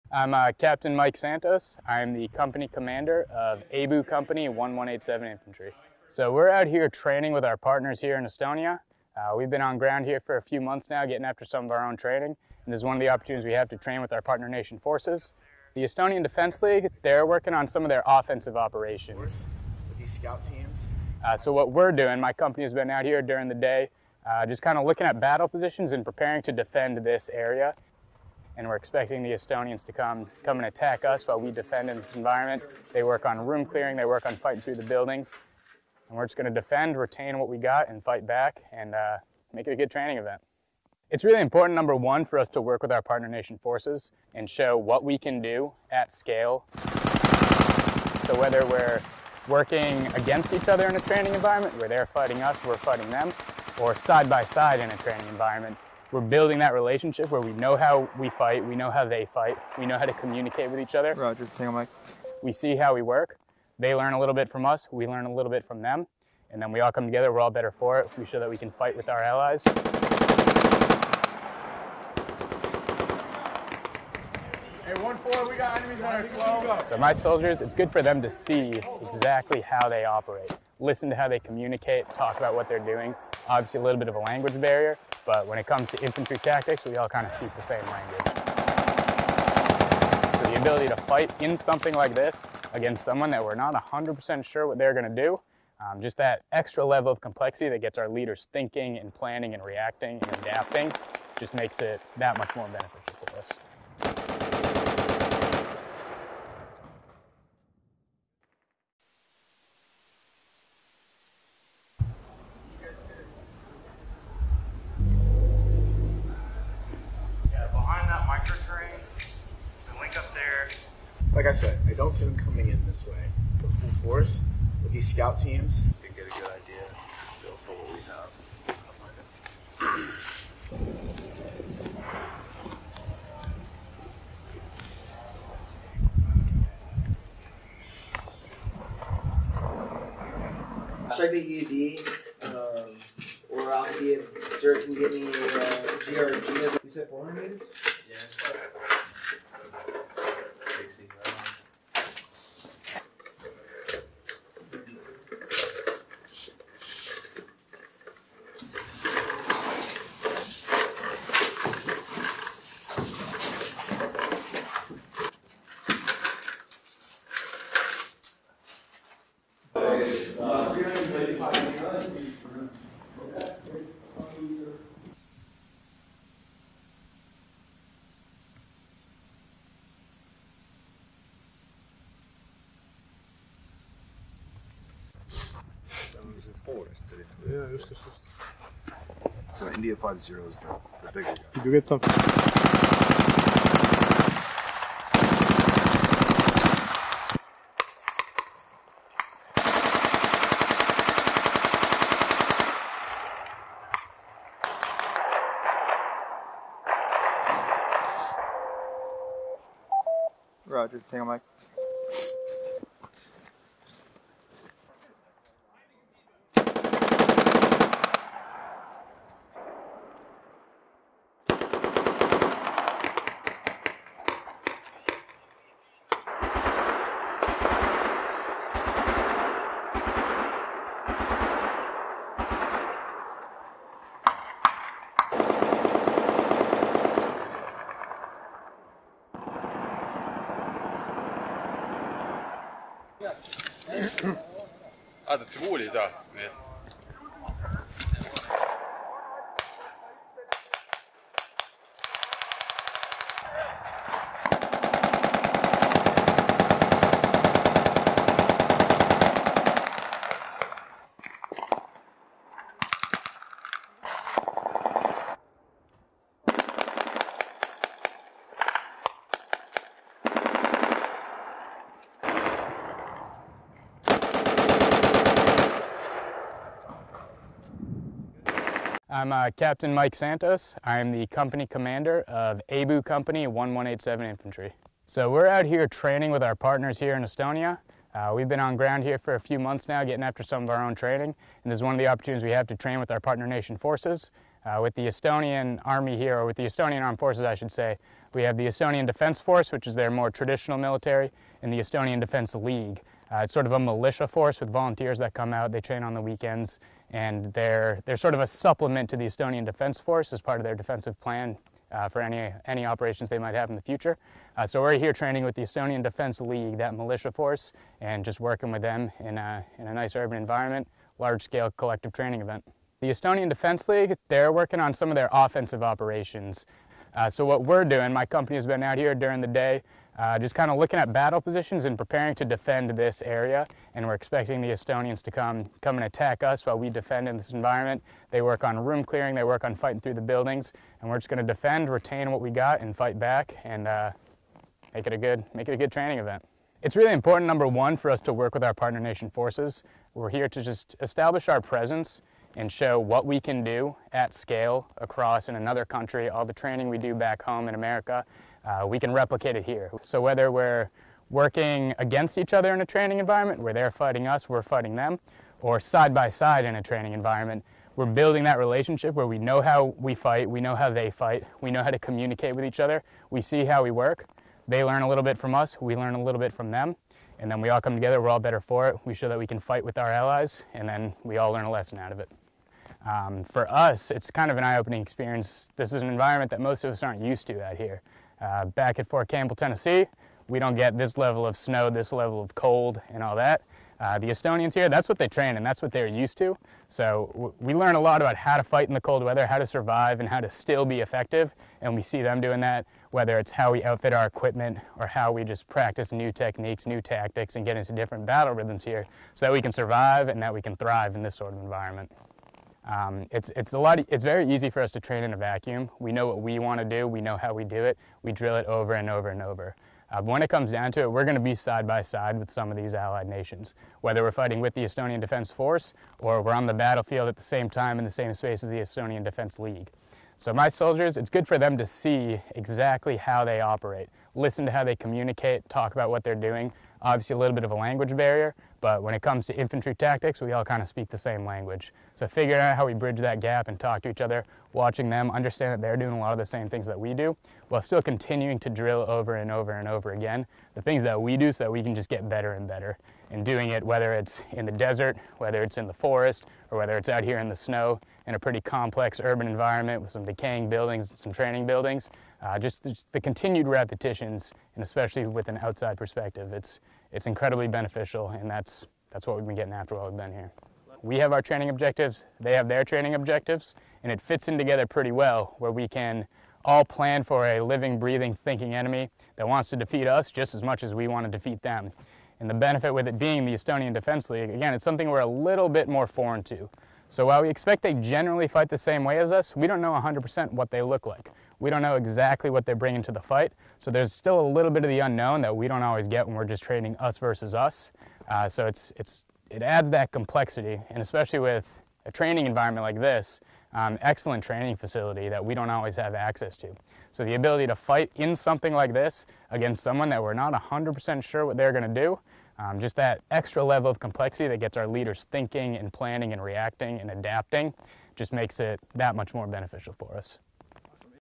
U.S. and Estonian Military Tactical Excellence and Interoperability Drills in Estonia
U.S. Army Capt. Mike Santos, the commander of Company A, 1st Battalion, 187th Infantry Regiment, "Leader Rakkasans," 3rd Infantry Brigade Combat Team, 101st Airborne Division (Air Assault), supporting 3rd Infantry Division, discusses the benefit of training alongside Estonian Defense League Soldiers during a situational training exercise held in Estonia’s Central Training Area near Camp Tapa, Estonia, Jan. 28, 2024.

U.S., Estonian military drills demonstrate tactical expertise and interoperability in Estonia’s Central Training Area
CENTRAL TRAINING AREA, ESTONIA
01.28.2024

 The 3rd Infantry Division’s mission in Europe is to engage in multinational training and exercises across the continent, working alongside NATO Allies and regional security partners to provide combat-credible forces to V Corps, America’s forward deployed corps in Europe. (U.S. Army video by Staff Sgt. Oscar Gollaz)

Film Credits: Video by Staff Sgt. Oscar Gollaz
112th Mobile Public Affairs Detachment